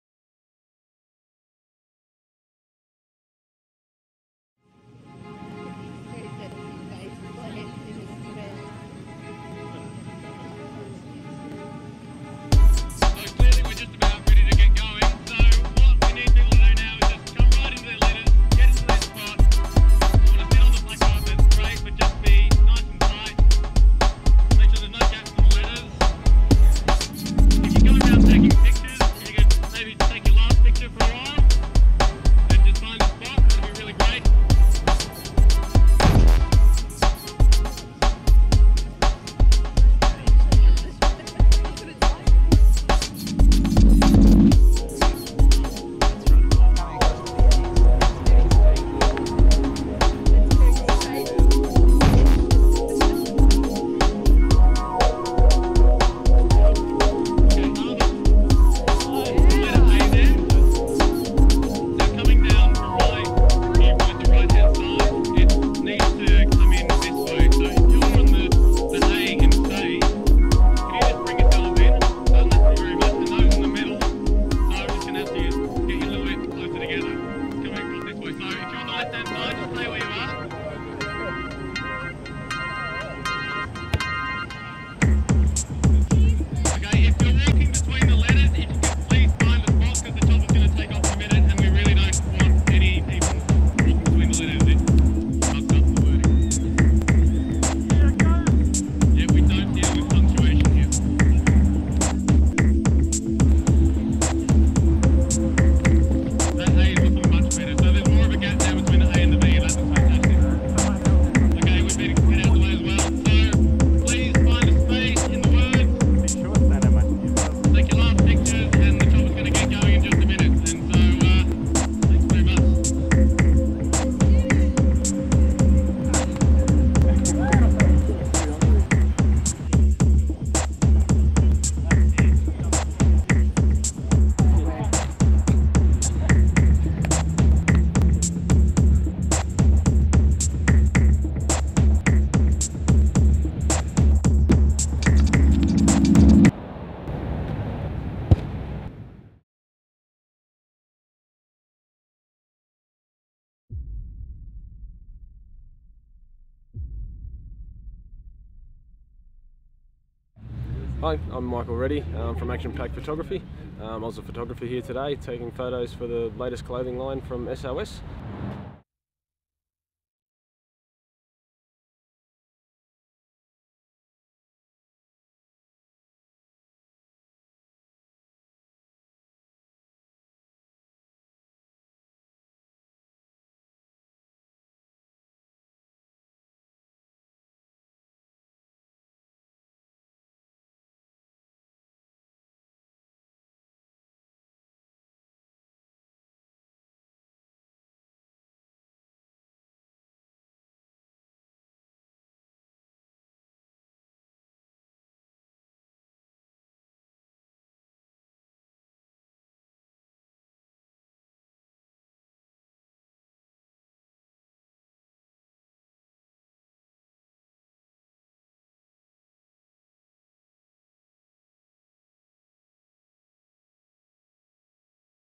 Men's Swimwear Australia: Men of Blackpool photo shoot 2010.mov
Men of Blackpool photo shoot for the world cup issue of GQ magazine.